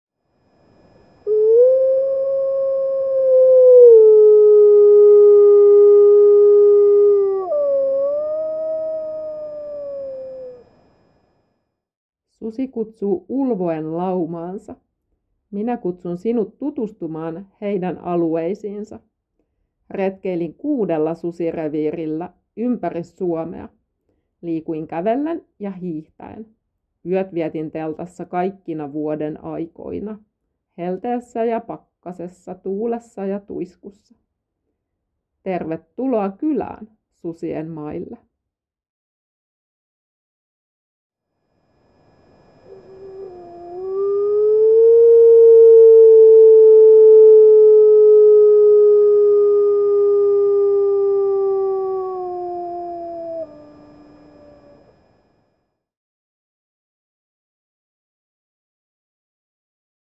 Mia Takula Susien mailla
Tervetuloa Susien maille. Mia Takula vaelsi, hiihti ja telttaili yli 150 yötä suomalaisilla susireviireillä selvittääkseen, millaista niillä on.

Toni Huhdan riistakameravideot on kuvattu susireviirillä ilman houkuttimia tai haaskaa. Videoita ei ole kuvattu kuvauskojusta."

Mia Takula: Susien mailla (Docendo 2020), sidottu, 272 sivua. ISBN: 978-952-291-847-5